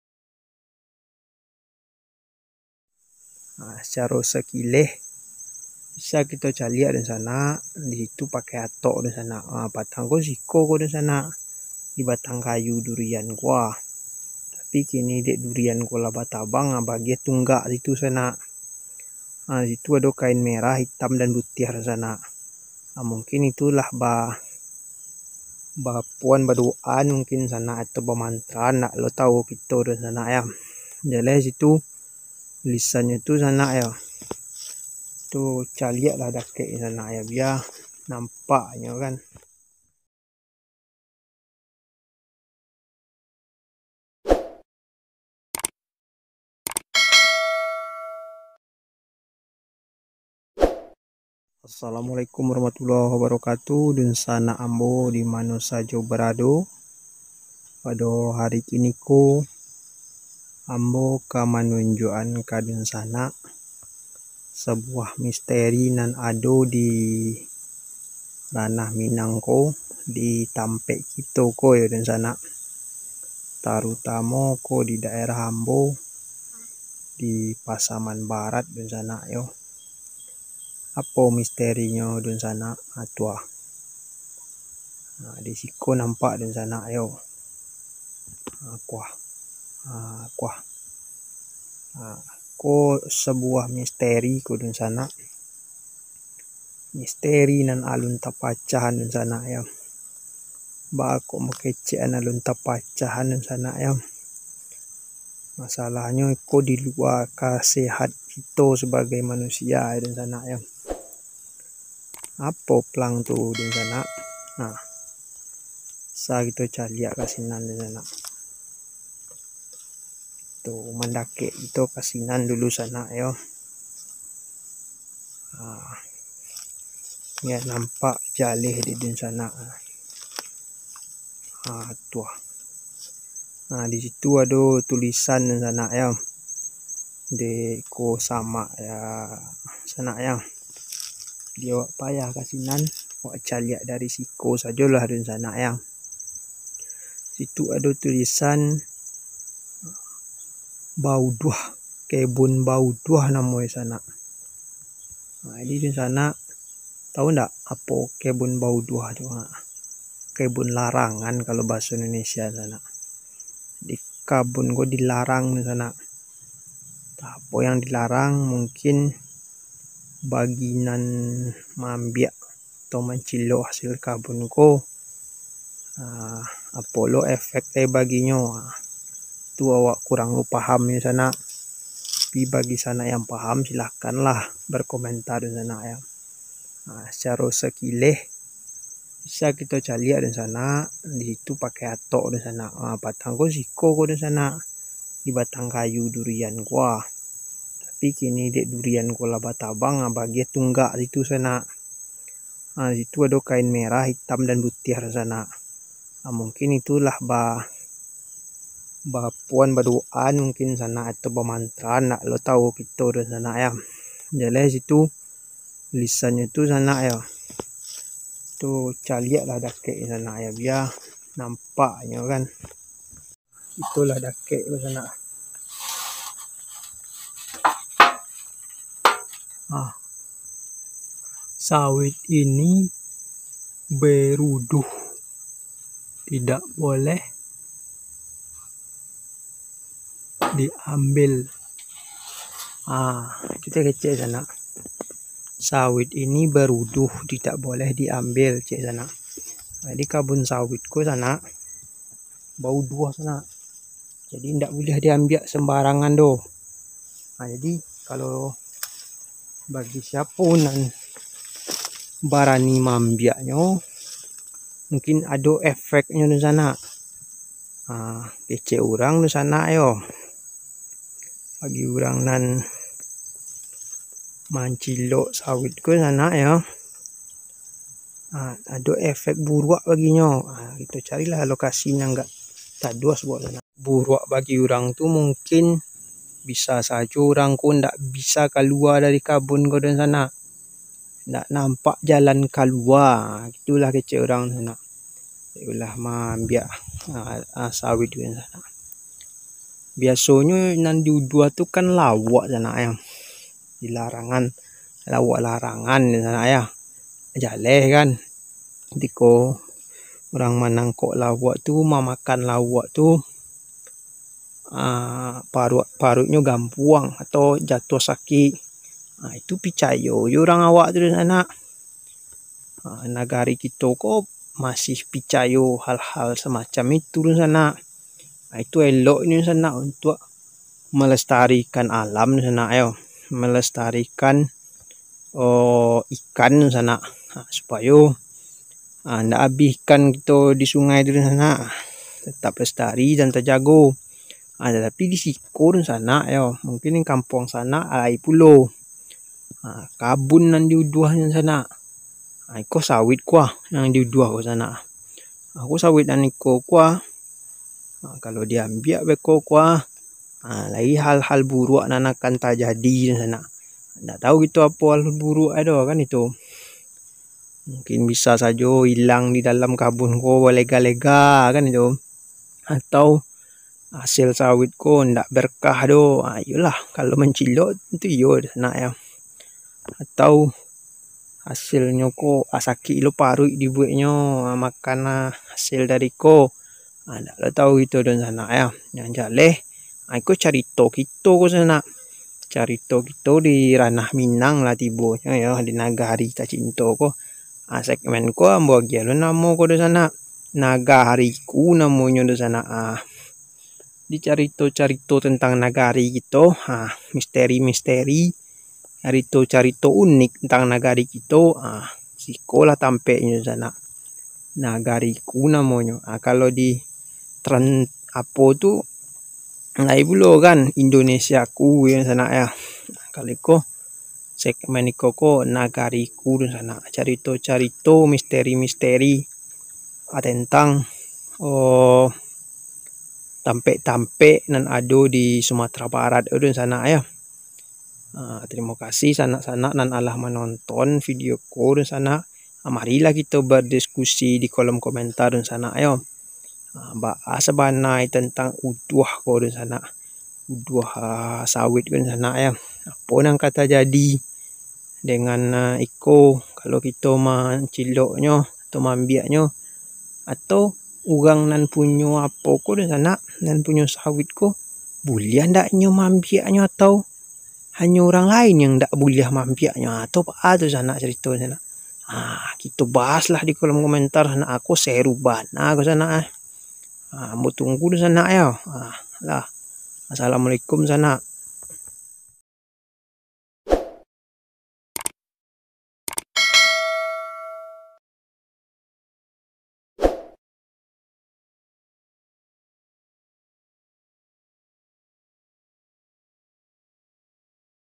MISTERI YANG BELUM TERPECAHKAN DI RANAH MINANG SUMATERA BARAT
MISTERI YANG ALUN TERPECAHKAN DI RANAH MINANG SUMATERA BARAT | NAGARIKU #1

Medsos Anak Sumbar